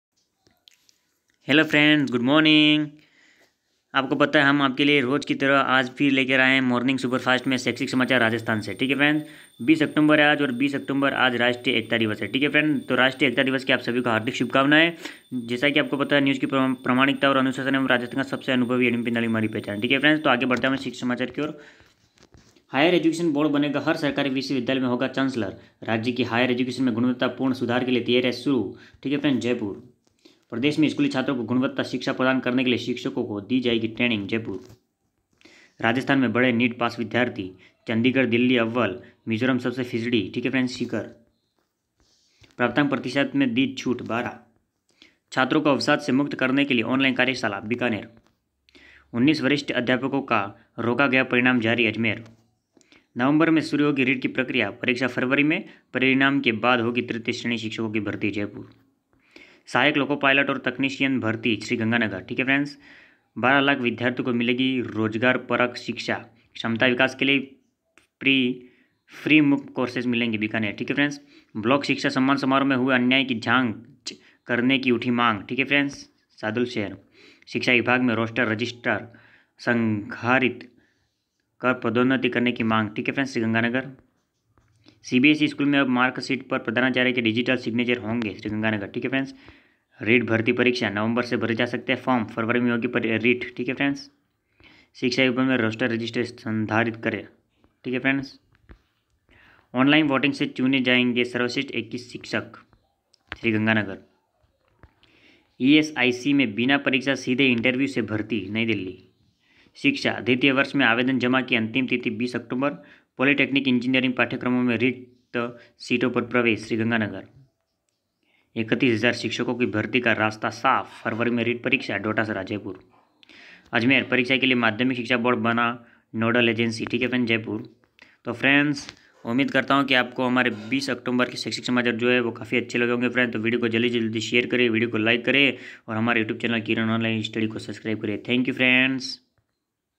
20 oct.2020 🗞शैक्षिक समाचार राजस्थान/राष्ट्रीय एकता दिवस/रीट भर्ती फॉर्म नवंबर में/ESIC no exam-KOS🙏
20 Oct.2020 के शिक्षा विभाग से जुड़ी हर खबर आप तक।।